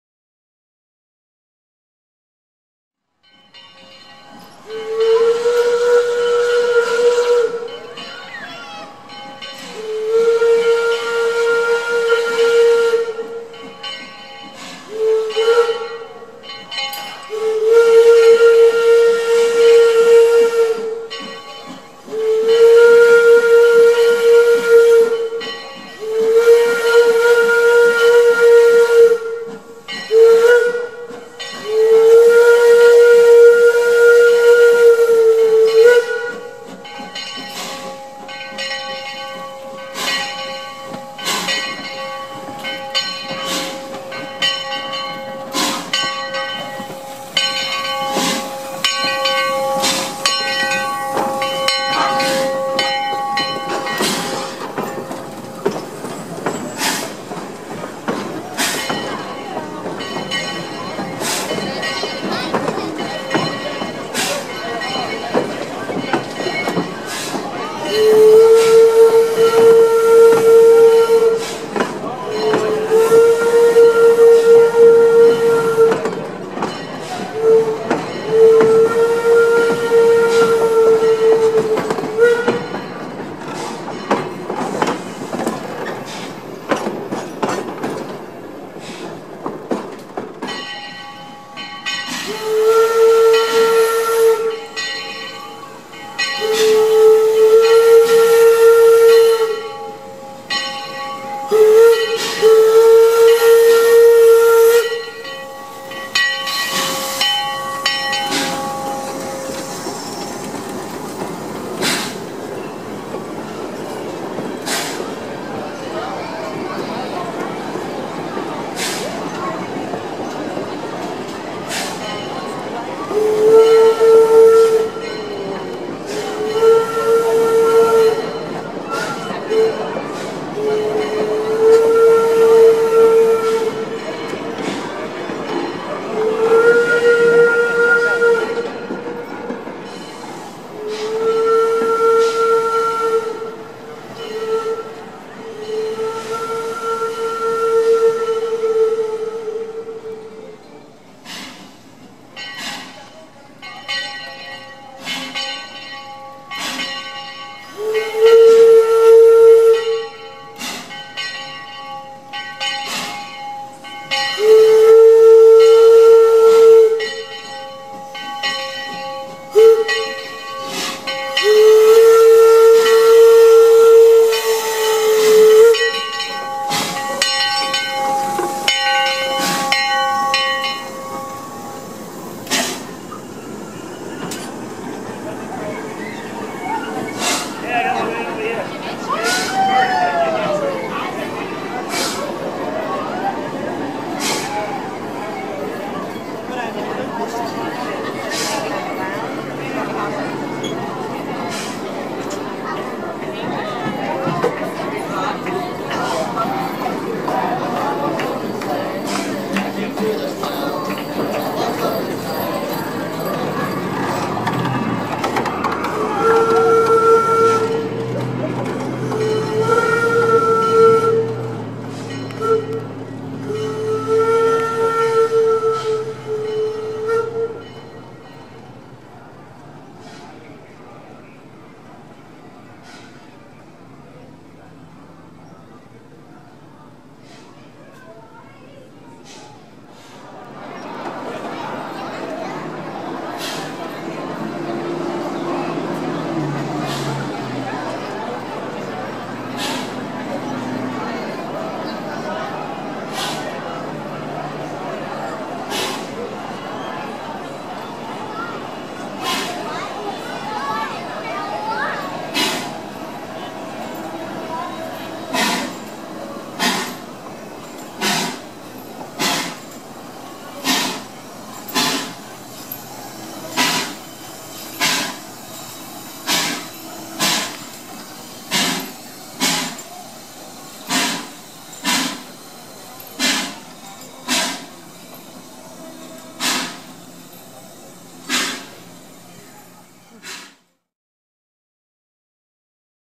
Orange Blossom Cannonball
Aside from the Walt Disney World Railroad, the only other steam operation in Florida is the Tavares, Eustis, & Gulf Railroad - more commonly known as The Orange Blossom Cannonball. Due to a track work project when I was there, the ride was reduced to about 1/2 mile long.  The railroad gave rides back and forth and offered free tickets to ride the entire line next season which I thought was very kind.  The upside to this was that it didn't take long to chase the train!